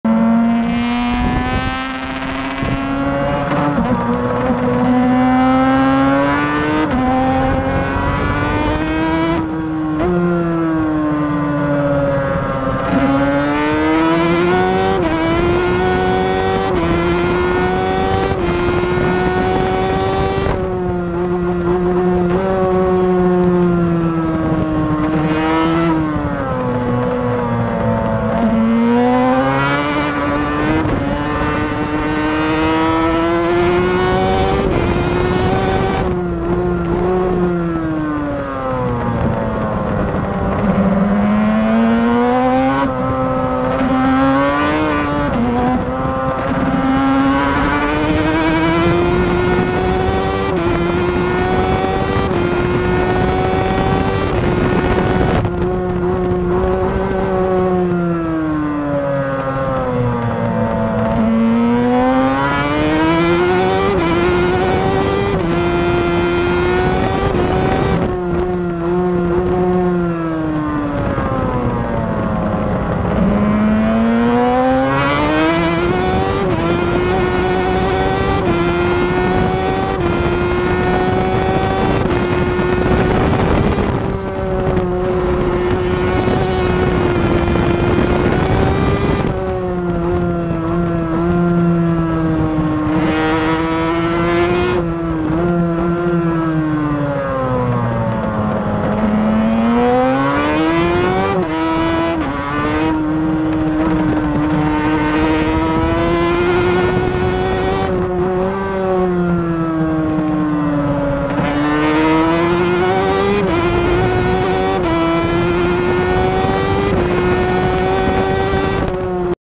Suter 500 Fabrizio Lai
On board rear view camera Misano Circuit
Fabrizio Lai